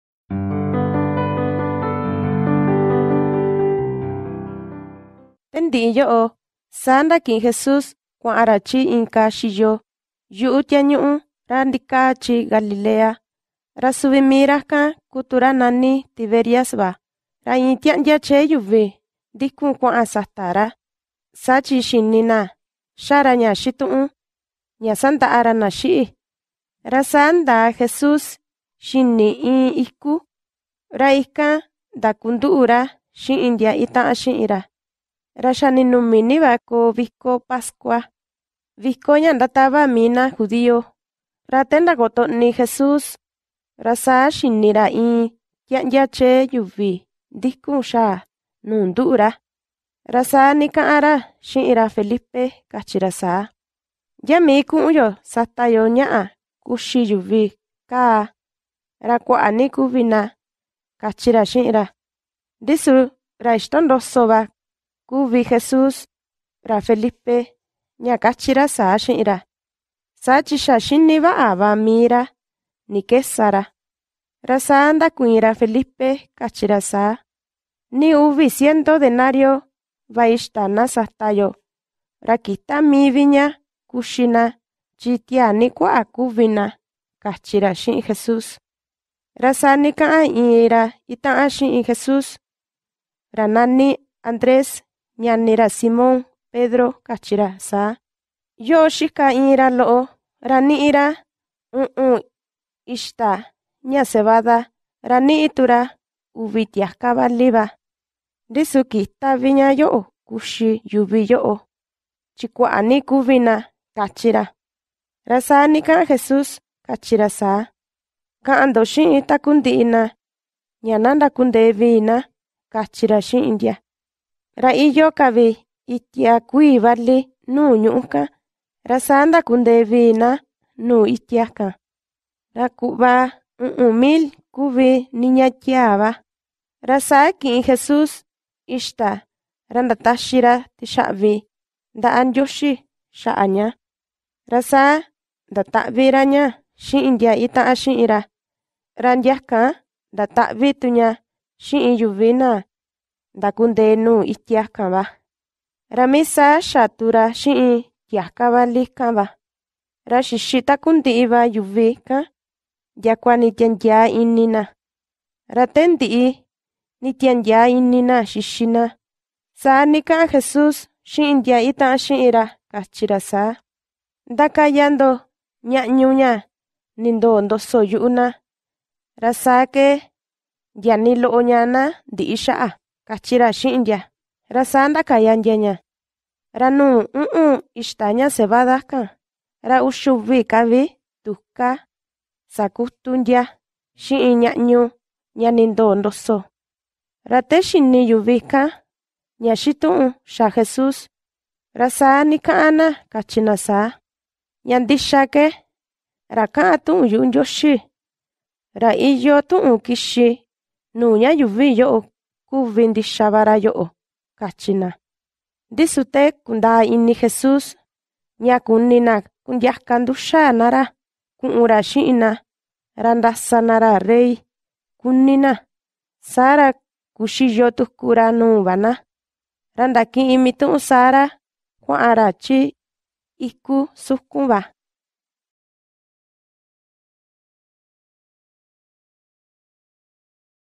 Mixteco de Metlatónoc y Cochoapa– John 6:1-15 - Jesus feeds a multitude [mxv]
Selection from the Gospel of John in the Mixteco de Metlatónoc y Cochoapalanguage of Mexico [mxv]